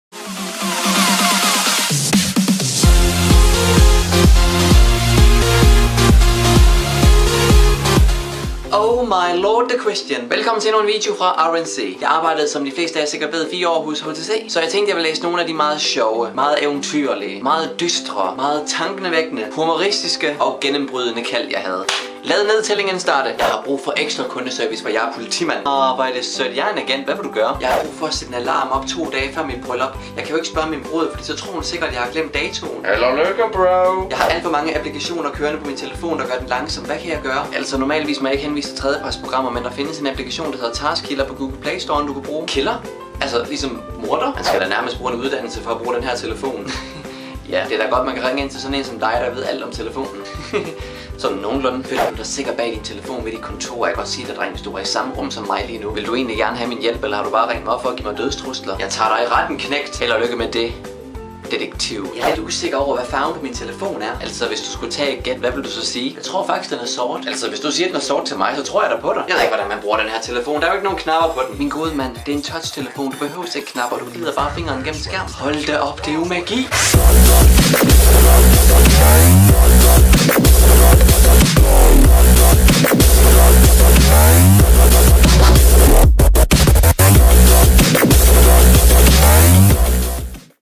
HTC Call Centre
BESKRIVELSE: Skitser fra hverdagens problemer! I dag ser vi på livet fra HTC kunde service centret!

1154 København K
Attn.: R&C